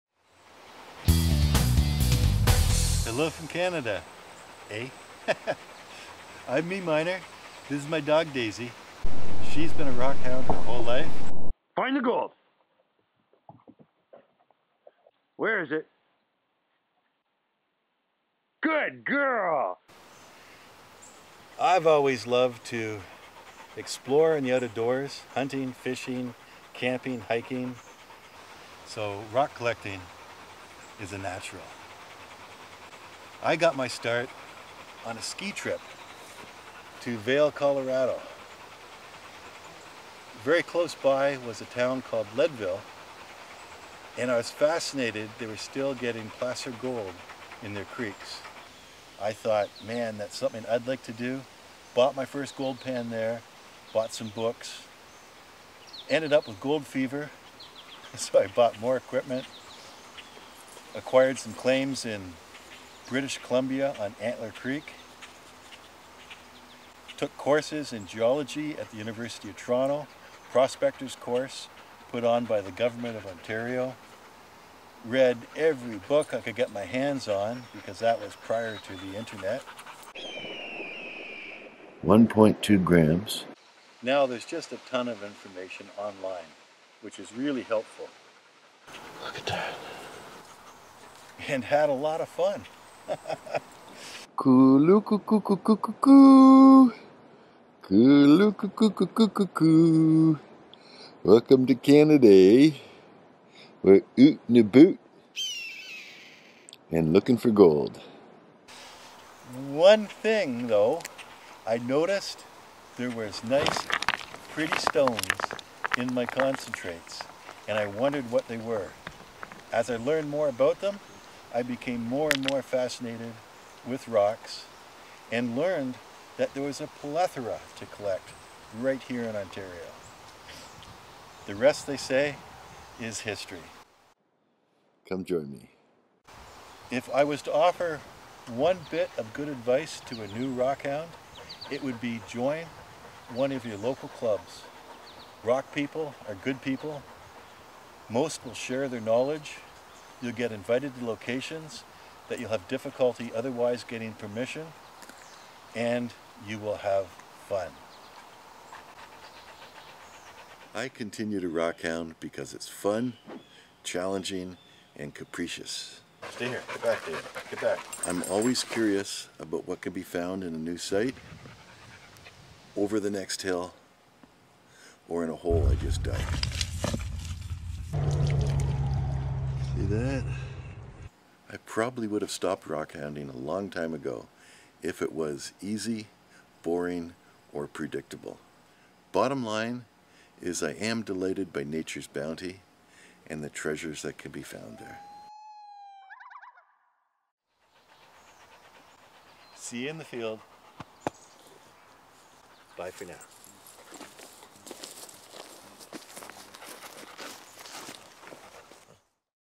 Why meMiner is a Rockhound
How did meminer get into rockhounding and why is he still a rockhound?Here is the answer provided to Youtuber KatyDid ROCKS! in a video with Quest for Details, MamLambo, Agate Angler, Theo Kellison and Michael VanDyke.